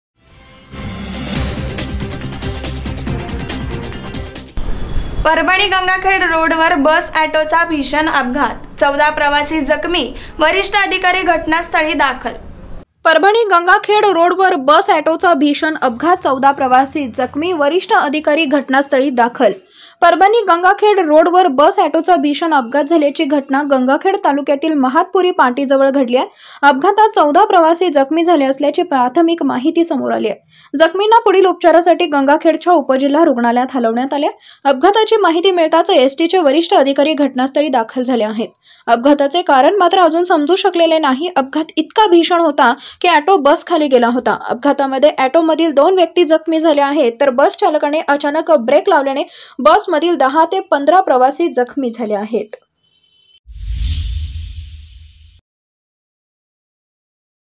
PARBHANI | गंगाखेड रोडवर बस - ऑटोचा भीषण अपघात, 14 प्रवासी जखमी, वरिष्ठ अधिकारी घटनास्थळी दाखल
SBN MARATHI
परभणी गंगाखेड रोडवर बस - ऑटोचा भीषण अपघात,  14 प्रवासी जखमी, वरिष्ठ अधिकारी घटनास्थळी दाखल